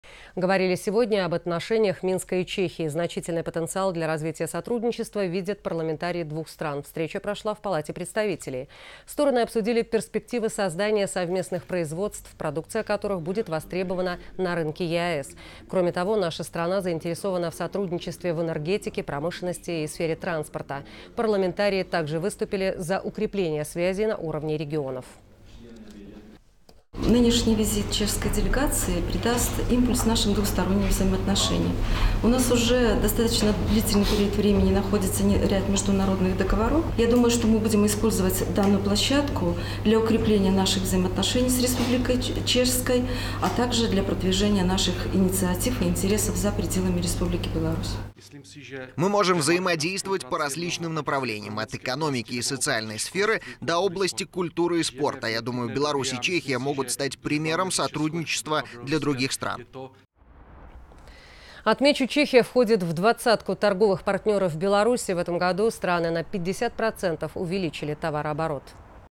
Парламентарии Беларуси и Чехии видят потенциал в развитии отношений
Встреча прошла в Палате представителей. Стороны обсудили перспективы создания совместных производств, продукция которых будет востребована на рынке ЕАЭС. Кроме того, наша страна заинтересована в сотрудничестве в энергетике, промышленности и сфере транспорта.